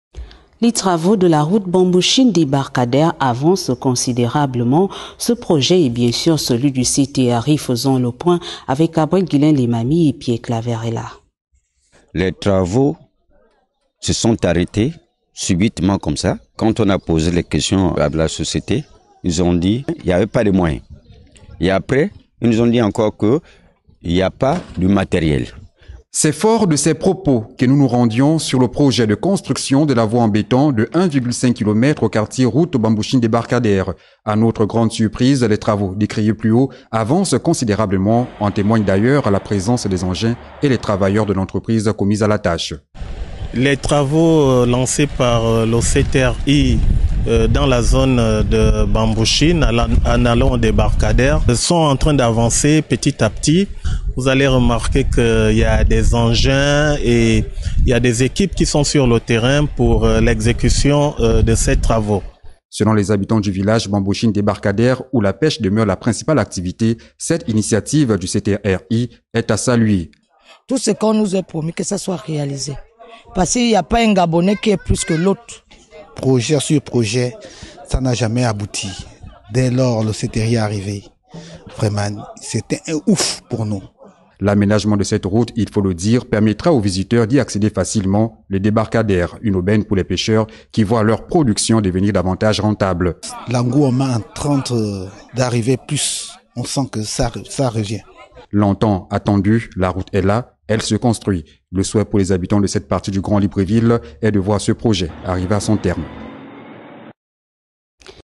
Travauxpublics : la route Bambouchine - Débarcadère bientôt livrée aux usagers
Sous l'égide du CTRI,  les travaux sur l'ace Bambouchine/ débarcadère ont repris.
Le projet était à l'arrêt grâce à l'avènement du CTRI, le projet de construction de voie à béton Bambouchine - Débarcadère avance bien. et la population est satisfait.